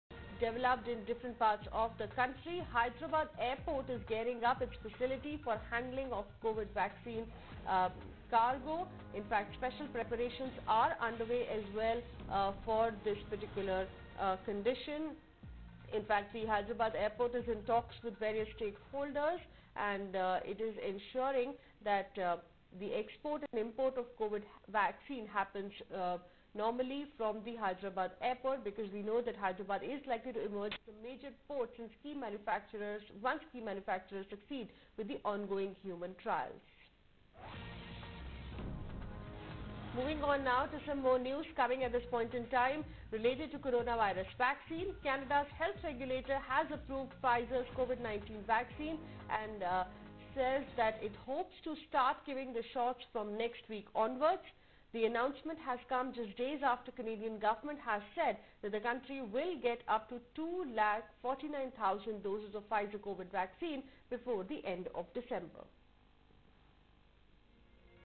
RGIA prepares for export & import of COVID-19 vaccines
GMR Hyderabad Air Cargo is equipped with temperature controlled facilities that ensure an unbroken cool-chain for global vaccine shipments.